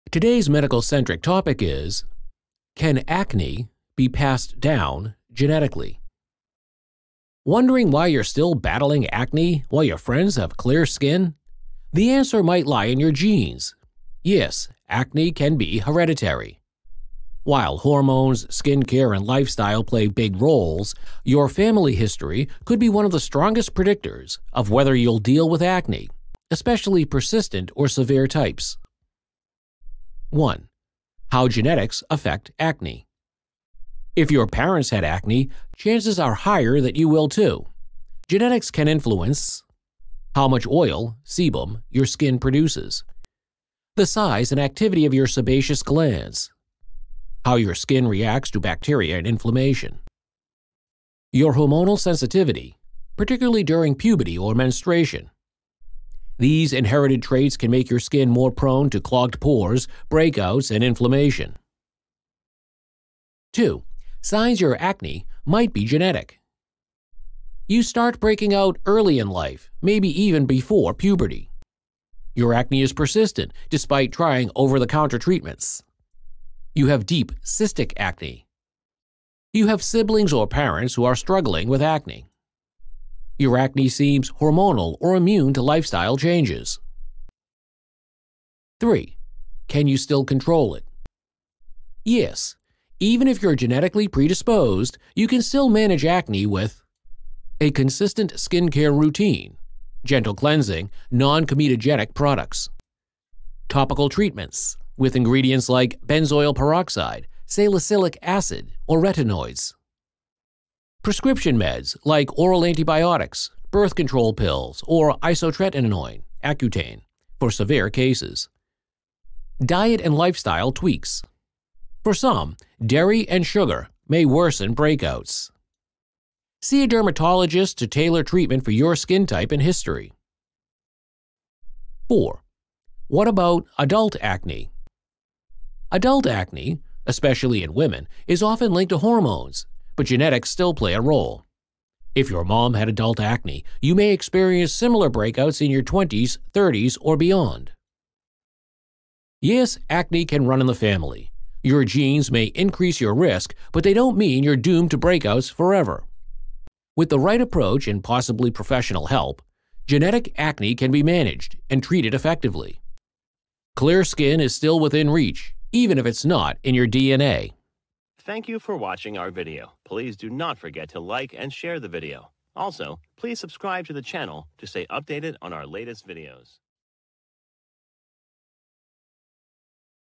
Is Acne In Your DNA? The Truth About Genetic Acne
Acne is one of the most common skin conditions in the world, affecting millions regardless of age. But if you've ever wondered, “Why do I have acne even though I take care of my skin?”, the answer might be written in your DNA. Yes, genetics can play a major role in your likelihood of developing acne.
🔬 The Genetic Link to Acne

Research shows that if your parents struggled with acne, there’s a higher chance you will too. In fact, a 2002 study published in the Journal of Investigative Dermatology found that individuals with a family history of acne were four times more likely to develop it themselves. This is because genes can influence how your skin produces oil (sebum), how quickly your skin cells regenerate, and how your immune system reacts to inflammation or bacteria—three key players in acne development.
🧬 What Exactly Is Inherited?

You don’t inherit acne directly, but you may inherit traits that create a “perfect storm” for breakouts:

    Oily skin due to overactive sebaceous glands

    Slower cell turnover, leading to clogged pores

    Heightened inflammatory responses to bacteria like Cutibacterium acnes

    Hormonal imbalances, often triggered by puberty or stress, can also be inherited and influence breakouts

👨‍👩‍👧‍👦 Nature vs. Nurture

While genetics can predispose you to acne, lifestyle still plays a massive role. Diet, stress, skincare habits, and sleep all influence your skin health. Even if you’re genetically prone, the right routine and possibly dermatologist-prescribed treatments can help manage it effectively.
🧴 Can You Outsmart Genetic Acne?

Yes, to an extent. Here are a few tips:

    Use non-comedogenic (non-pore-clogging) products

    Stick to a gentle cleanser and moisturizer routine

    Consider topical treatments like retinoids or benzoyl peroxide

    Don’t underestimate the power of a good dermatologist

    If hormonal acne is suspected, hormonal treatments like oral contraceptives or spironolactone might help (under medical supervision)